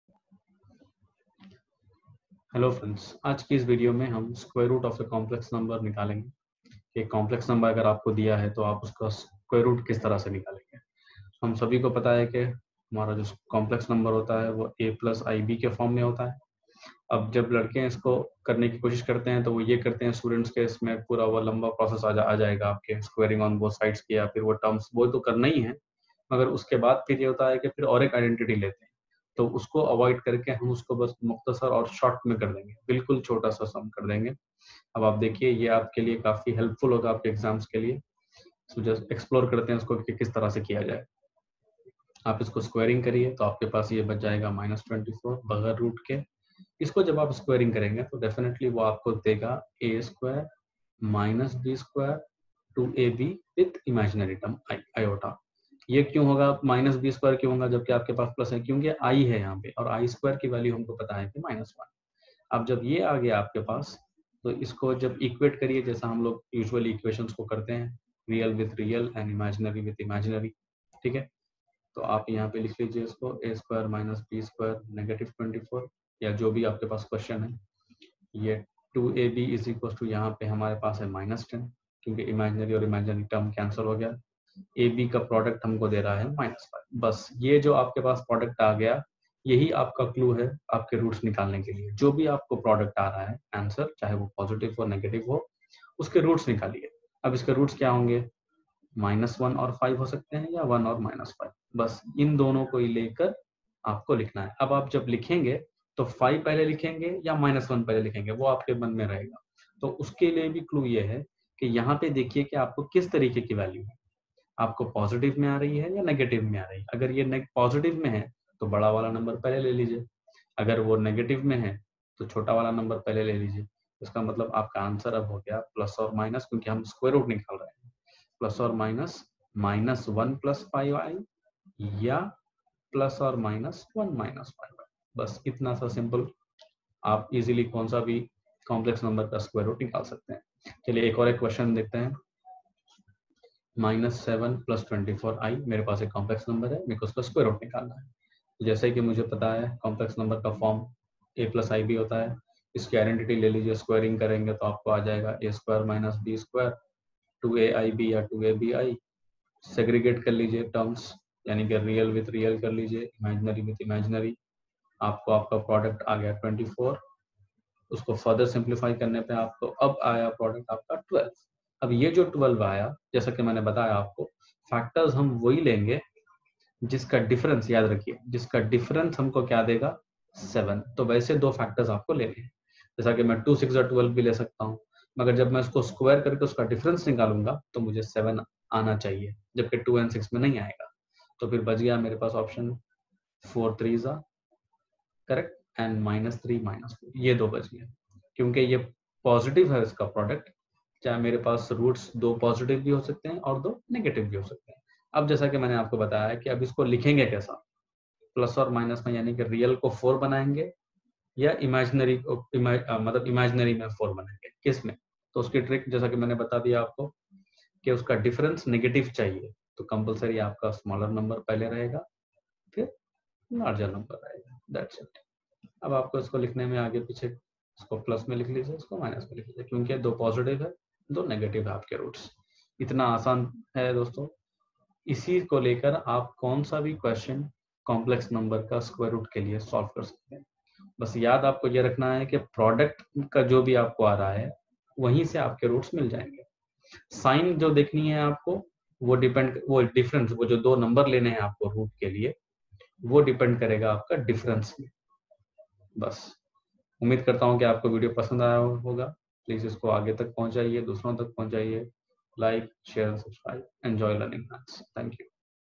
Square root of Complex number(Hindi)
short cut trick to workout complex number in seconds...